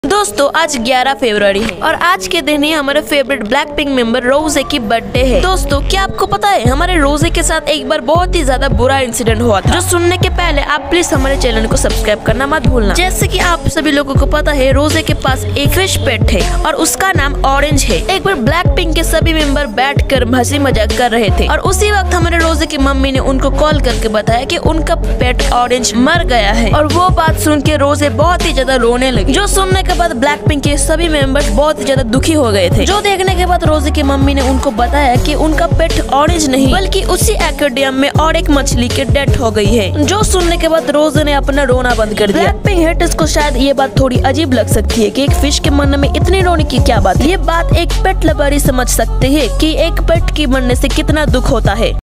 Blackpink Rose Cried Hearing Her Pet Fish Died.. 😞😭[ blackpinkrose blackpinkfacts kpopindia]
Blackpink Rose Cried Hearing Her Pet Fish Died.. 😞😭

Army's If You Like To Watch Our Videos.. 😊
regular kpop And Bts Updates.. 😊💜